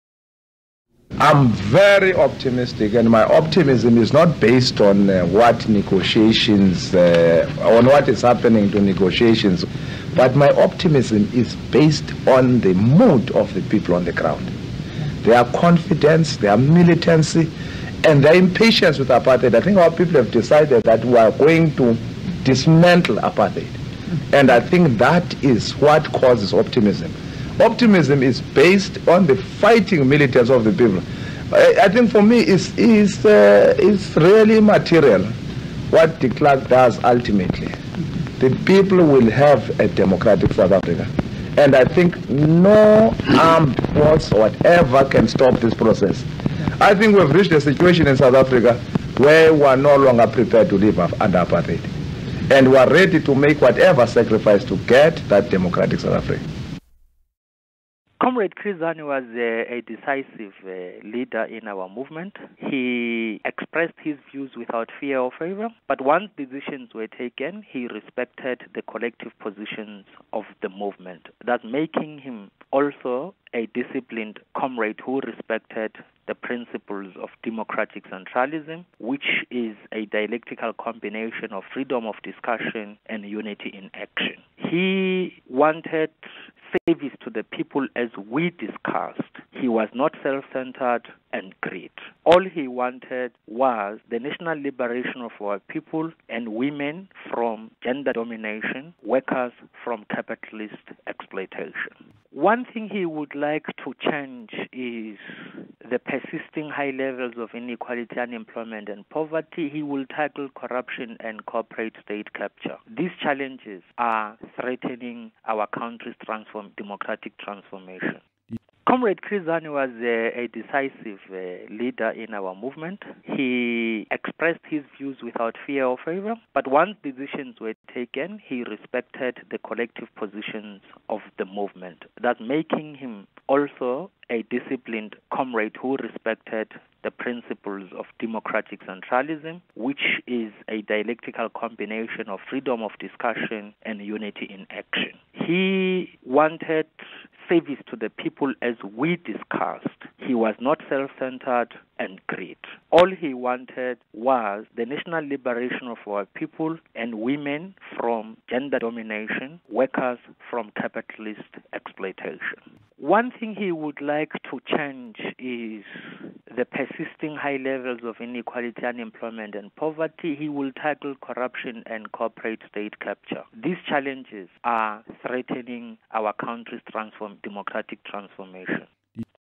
Chris Hani statements still apply 24 years after1
Chris Hani, 24 years after he was shot dead in South Africa. Prophetic statements he made before his death in 1993. Compiler: Sandra Mulder